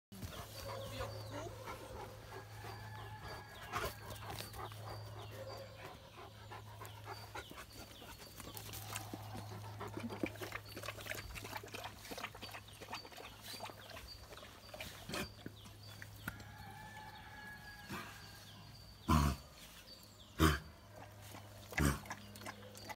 5. März 2019
Bernhardiner John Boy bei einer Vergessellschaftung